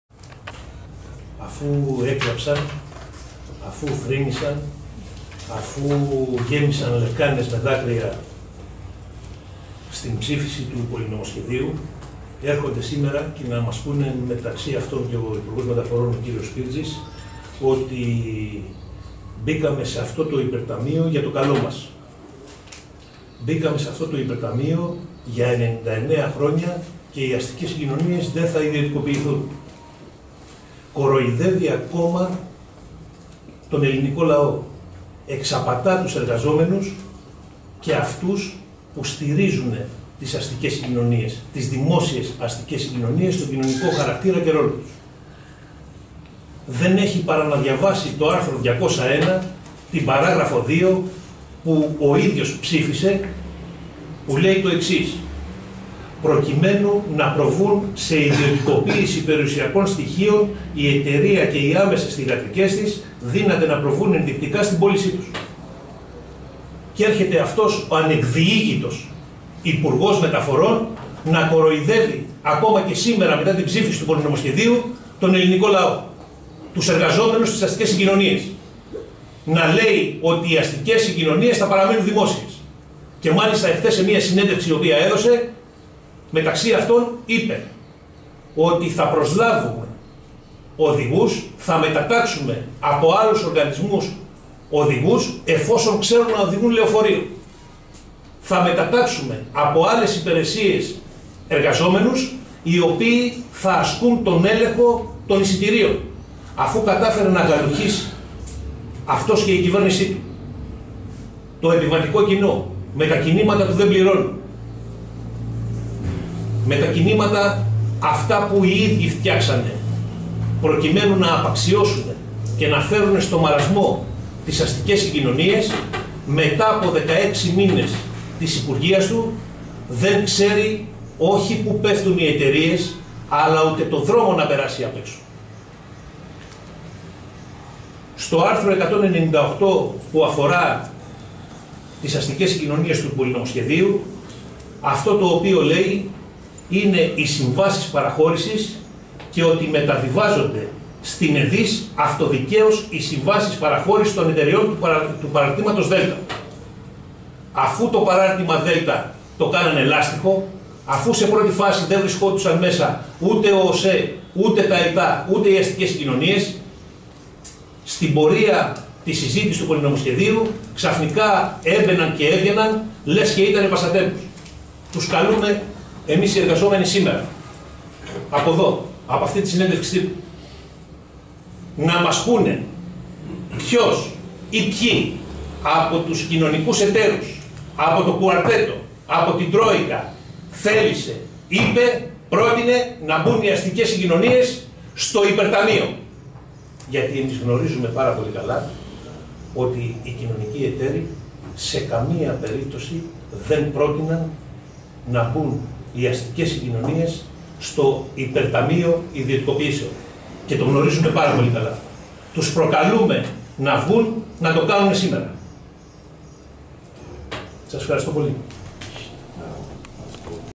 "Ο υπ. Μεταφορών κοροϊδεύει τους εργαζομένους"
Ο πρόεδρος των τεχνικών ΟΑΣΑ - ΟΣΥ Απόστολος Ραυτόπουλος "επιτίθεται" εναντίον του υπουργού Μεταφορών Χρήστου Σπίρτζη για την ένταξη των αστικών συγκοινωνιών στο υπερ-Ταμείο Ιδιωτικοποιήσεων, υποστηρίζοντας - μάλιστα - ότι αυτό δεν συνέβη, κατόπιν πρότασης των δανειστών.